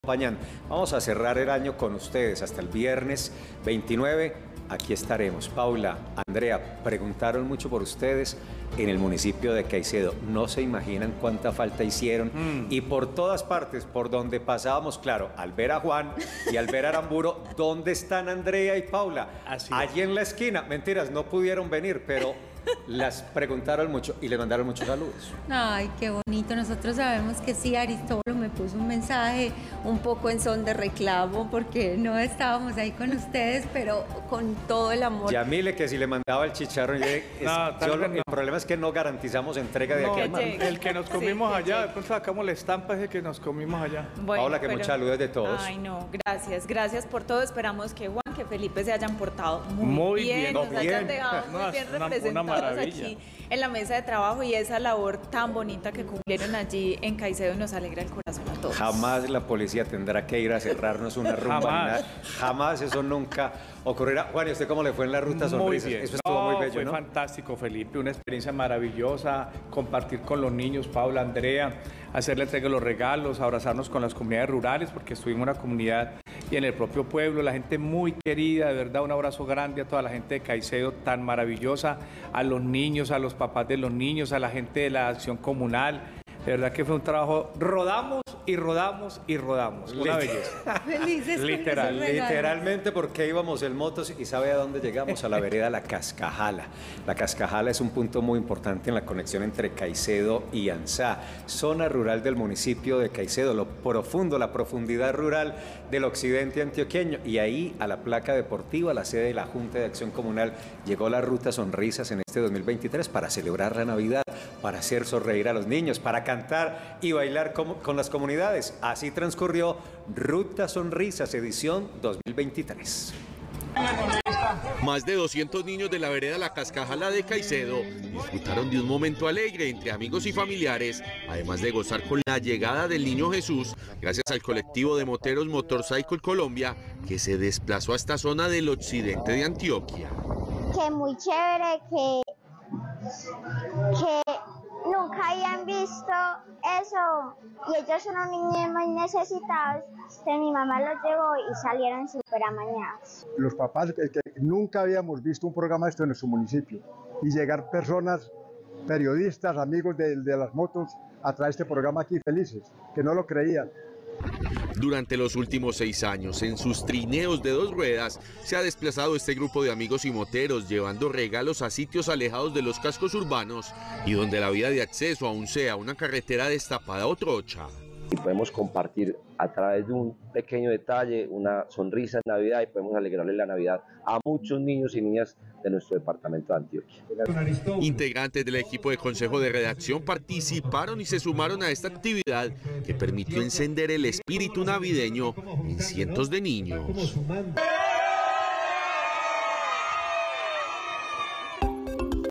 Nos fuimos con la Ruta Sonrisas para el Municipio de Caicedo Antioquia
¡Hola a todos los seguidores de nuestro canal! Hoy queremos compartir con ustedes una noticia maravillosa: ¡la RutaSonrisas ha vuelto a dejar su huella en los corazones de los niños y niñas de las montañas de Antioquia! Esta vez, llegamos hasta la Vereda La Cascajala del Municipio de Caicedo, llevando alegría y felicidad a cada pequeño que encontramos en el camino.

Fue una experiencia emocionante y llena de amor, ver las sonrisas en los rostros de los niños al recibir sus regalos y compartir momentos inolvidables con ellos. Sabemos que para muchos de ellos, estos regalos son los únicos que recibirán en estas navidades, por lo que nos llena de felicidad poder llevarles un poco de ilusión y hacerlos sentir especiales.

Queremos agradecer a todas las personas que hicieron posible esta hermosa jornada: a nuestros patrocinadores, voluntarios y a todos los que se unieron a esta iniciativa de llevar sonrisas a los niños de las zonas más apartadas de nuestro país. Gracias por ser parte de esta gran familia de RutaSonrisas y por ayudarnos a dejar un impacto positivo en la vida de estos pequeños.

Sigamos trabajando juntos para llevar alegría y esperanza a quienes más lo necesitan. ¡Nos vemos en la próxima parada de la RutaSonrisas! ¡Hasta pronto!